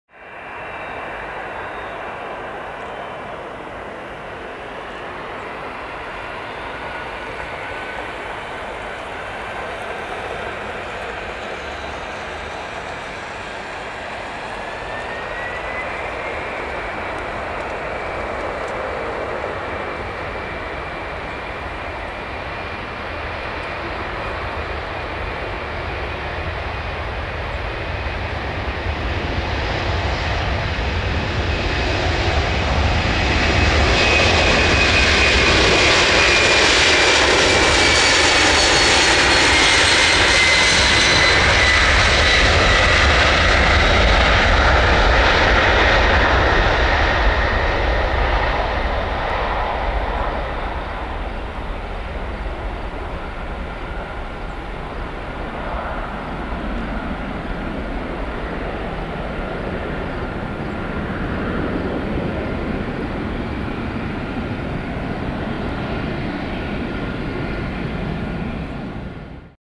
Condor (Janosch Livery) 767-300ER [D-ABUE] Takeoff Vancouver Airport (YVR)
Flight: CFG71 YVR-FRA